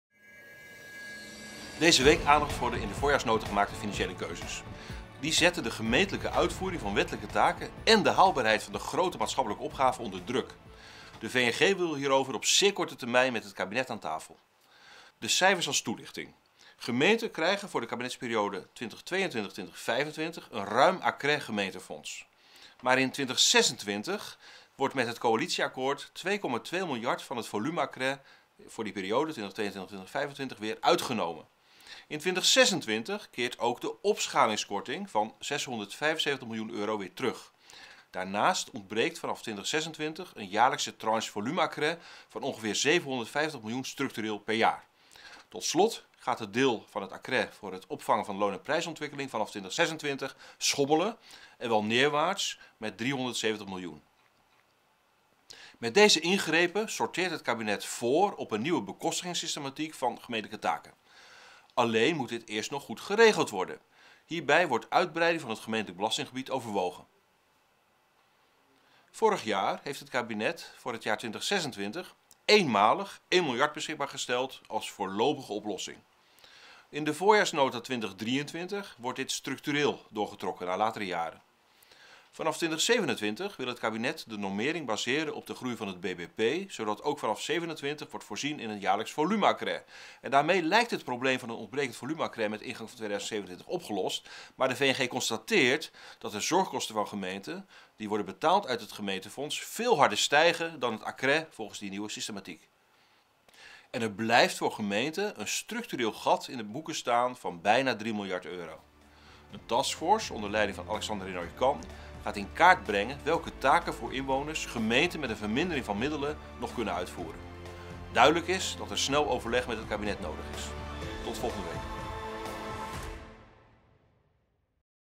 Gevolgen financiën gemeenten na Voorjaarsnota 2023 - Week Leonard Geluk 12 mei 2023
Deze week aandacht voor de in Voorjaarsnota gemaakte financiële keuzes. Die zetten de gemeentelijke uitvoering van wettelijke taken én de haalbaarheid van de grote maatschappelijke opgaven onder druk. De VNG wil hierover op zeer korte termijn met het kabinet om tafel. De cijfers in deze video als toelichting: